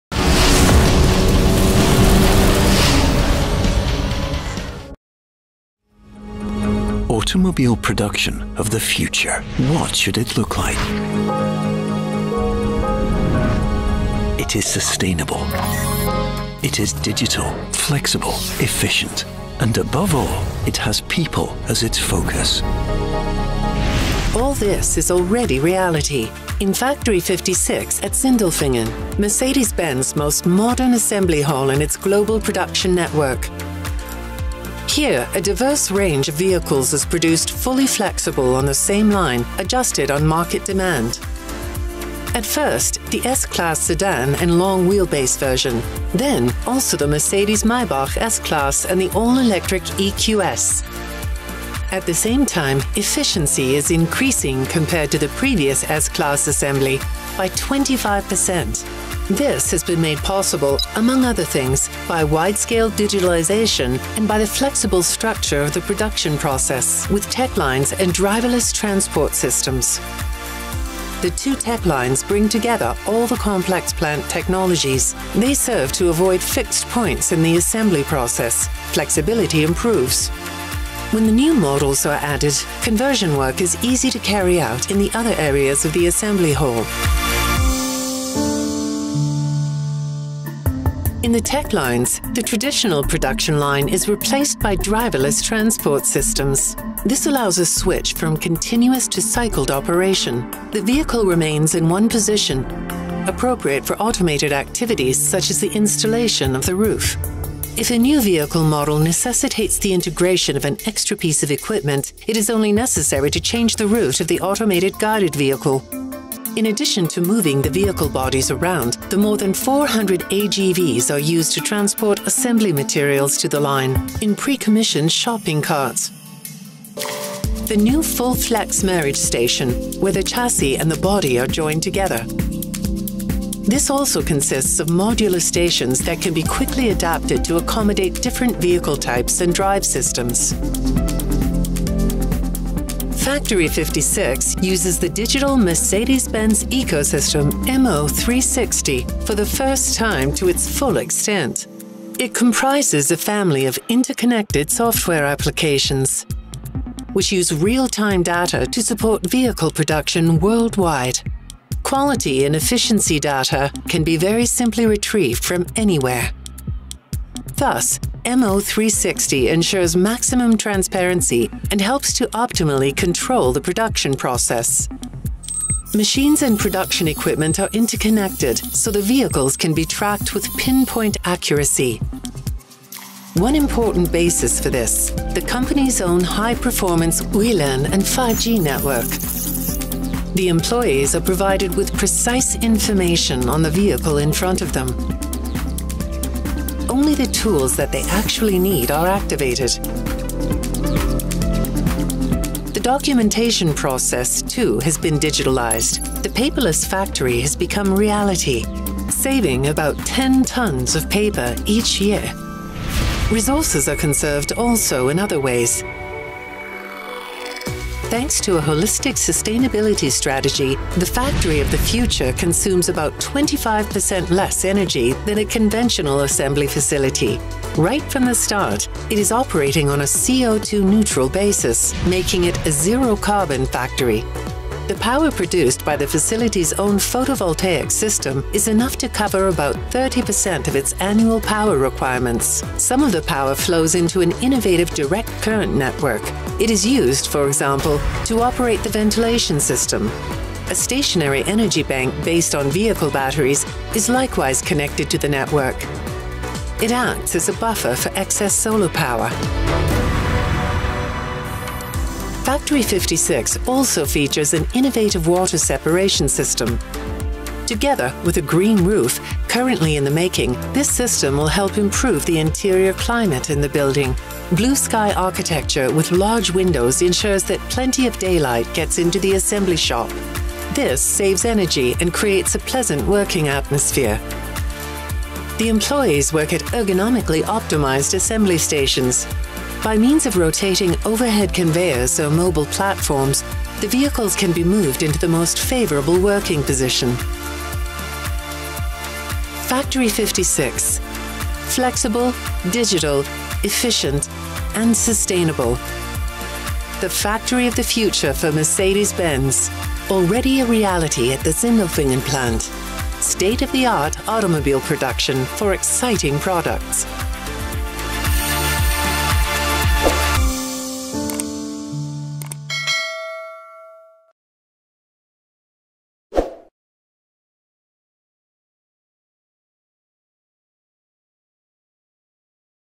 The Most Modern Car Factory built by Mercedes-Benz
Factory 56 embodies the future of production at Mercedes-Benz and sets new standards for the automotive industry. With an investment of approximately 730 million Euros, the factory at the Mercedes-Benz plant in Sindelfingen is a clear commitment to Germany as a business location. In total, Mercedes-Benz is investing about 2.1 billion euros at the Sindelfingen site. The most important feature of Factory 56 is maximum flexibility. On only one level, Factory 56 can perform all assembly steps for vehicles of different designs and drive systems – from conventional to all-electric drive. At first, the new generation of the Mercedes-Benz S-Class sedan and long-wheelbase version will drive off the production line in Factory 56. Later, the Mercedes-Maybach S-Class and the EQS, the first all-electric member of the new S-Class family, will also be produced on the same line. The assembly hall is 100 percent flexible, so that all Mercedes-Benz model series can be integrated into ongoing production in the shortest possible time, depending on demand – from compact vehicles to SUVs.